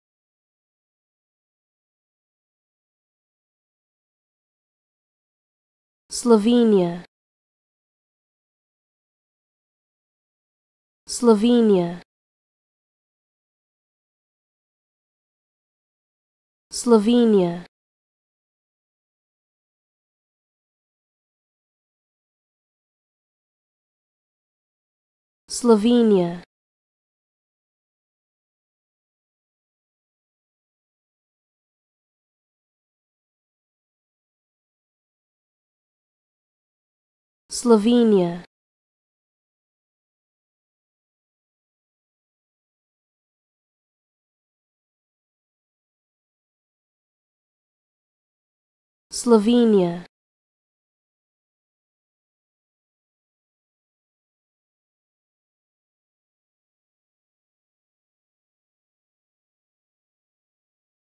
How To Say Slovenia
Learn how to say Slovenia with EmmaSaying free pronunciation tutorials.